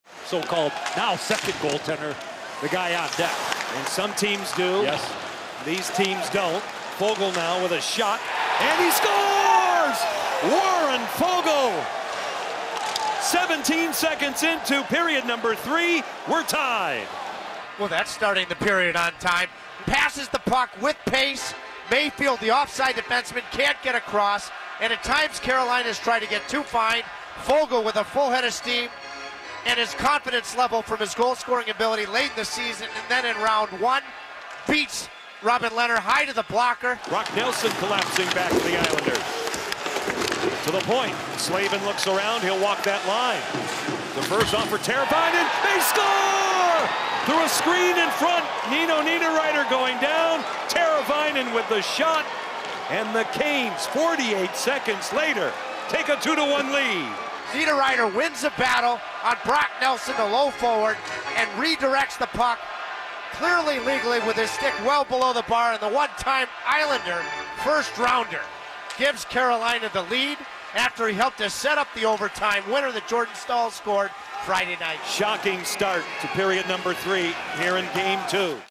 Hurricanes strike twice in 48 seconds to take a 2-1 lead
Warren Foegele gets a long pass off the boards from Lucas Wallmark and buries a shot from the circle to tie the game just 17 seconds into the 3rd period, then 48 seconds later, Nino Niederreiter makes a beautiful deflection to beat Robin Lehner and give the Hurricanes a 2-1 lead!

Feeling social?